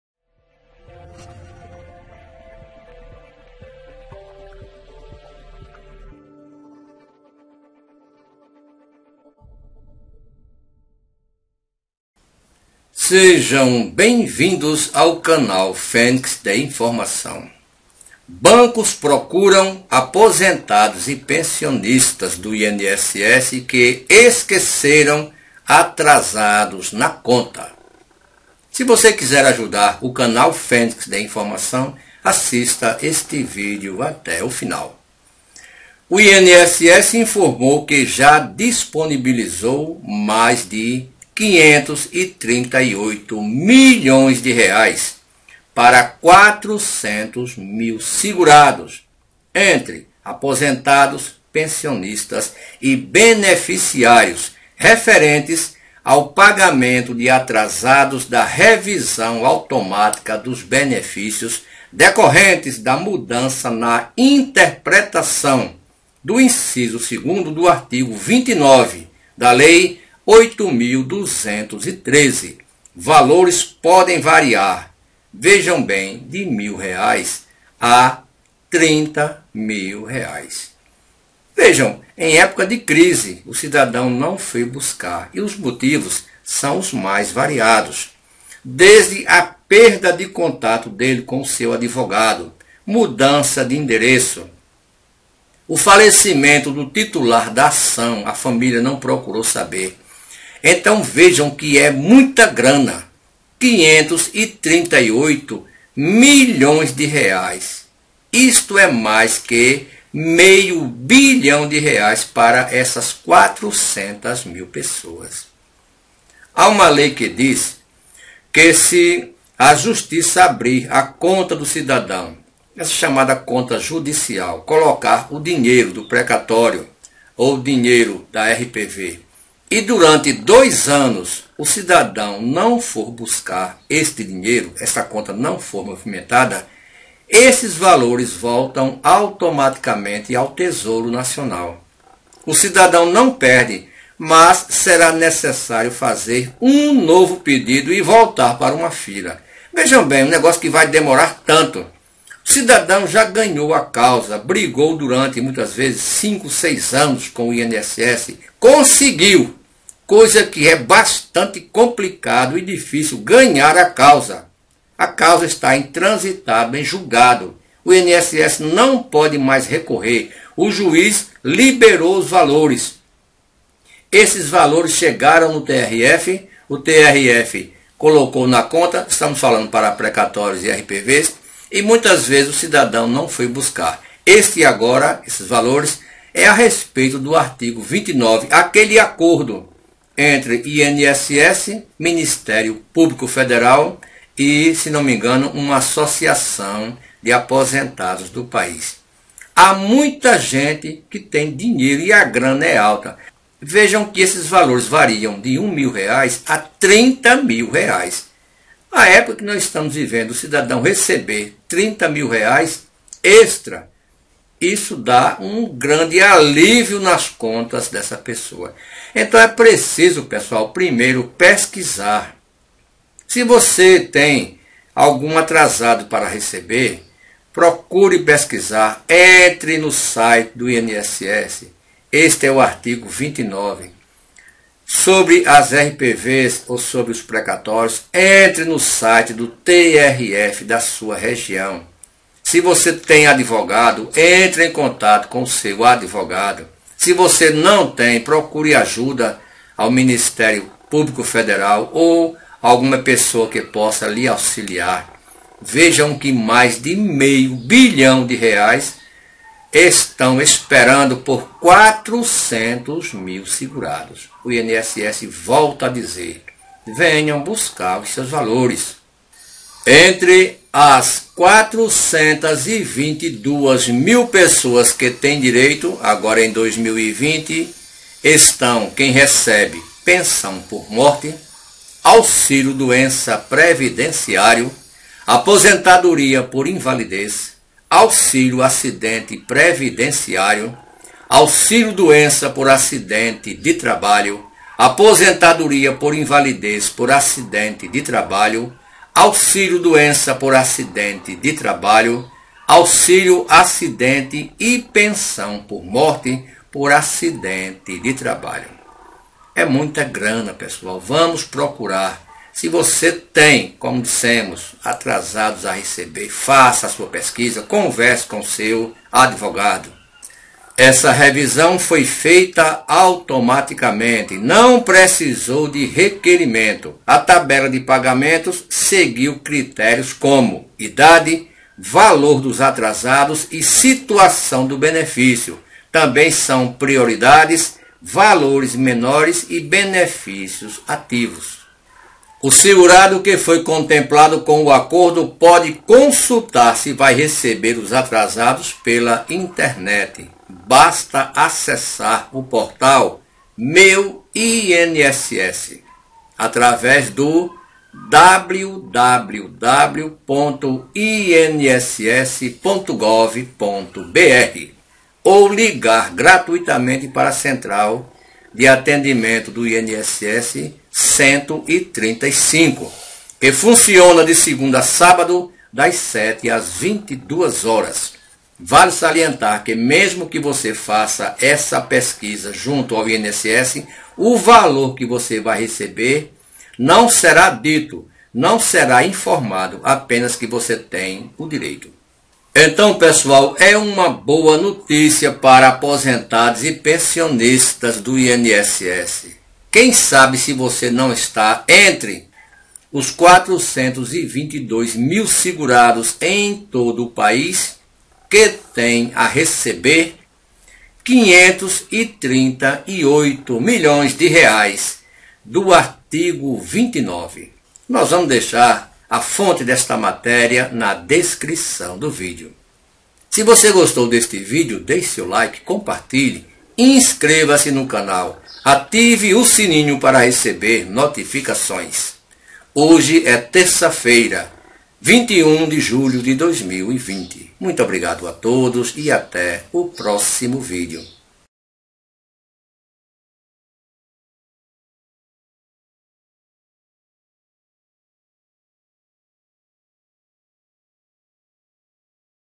BANCOS PROCURAM APOSENTADOS E PENSIONISTAS DO INSS QUE ESQUECERAM ATRASADOS NAS CONTAS
Bancos procuram aposentados e pensionistas do INSS que esqueceram atrasados na conta.

O INSS informou que já disponibilizou mais de R$ 538 milhões para 400 mil pessoas, entre aposentados, pensionistas e beneficiários referentes ao pagamentos de atrasadas da revisão automática dos benefícios decorrentes da mudança na interpretação do inciso II, do Artigo 29, da Lei 8.213.

Valores podem variar de R$ 1.000 a mais de R$30 mil.

Muitos aposentados podem não ter se dado conta da grana, e podem ter esquecido dinheiro no banco.

Por isso, é interessante consultar se tem alguma grana extra liberada para você.

No lote de 2020, o INSS já depositou o dinheiro nas contas dos beneficiários contemplados.